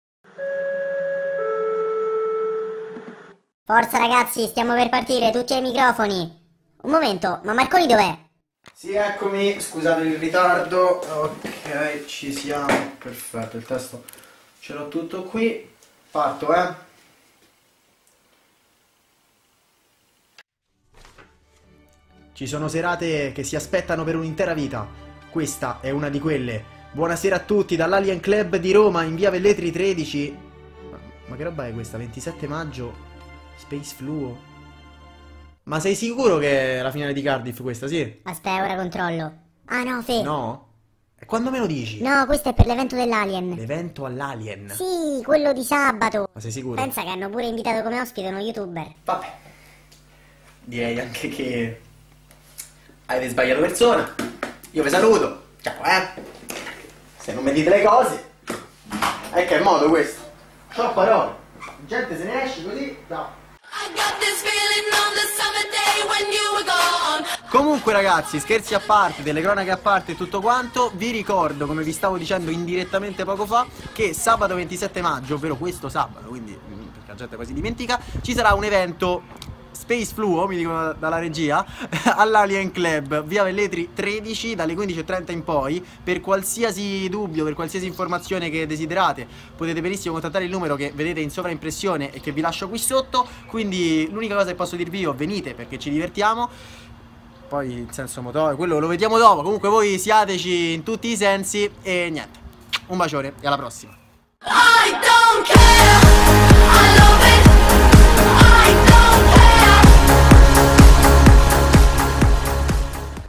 Un TELECRONISTA in DISCOTECA!! - 27|5|17
Come avrete capito dal video, questo sabato sarò ospite all'Alien Club di Roma, in Via Velletri 13, dalle 15:30 in poi.
Mi farebbe molto piacere che veniate NUMEROSI, anche perché SE LA TAJAMOOOOO!
Detto questo, per info e prenotazioni, contattate il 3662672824.